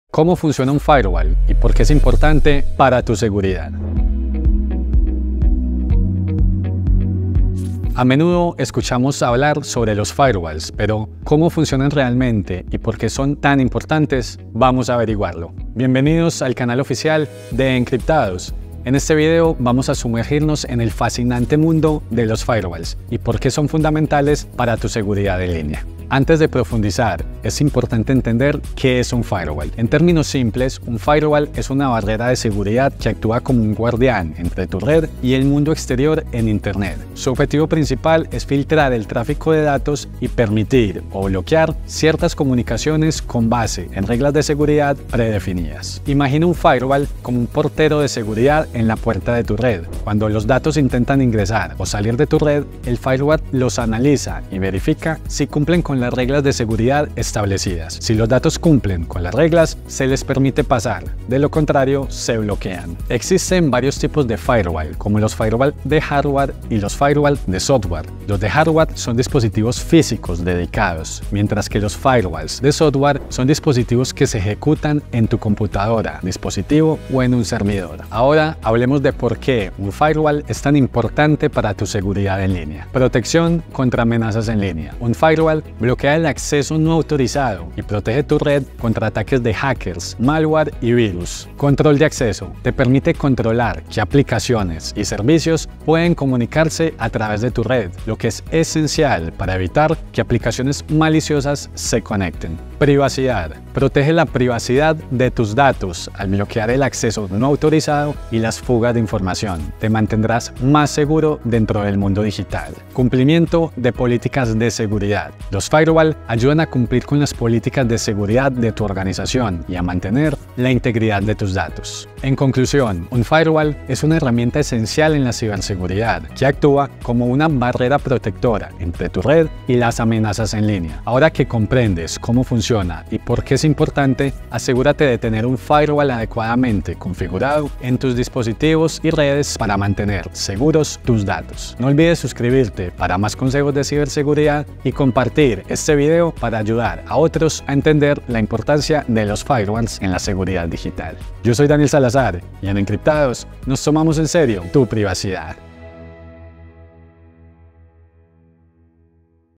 Firewall 🛡️ ¿Cómo Funciona y por Qué es Importante para tu Seguridad?
A menudo escuchamos sobre los firewalls 🛡️, pero ¿cómo funcionan realmente y por qué son tan importantes? ¡Vamos a averiguarlo! 🔐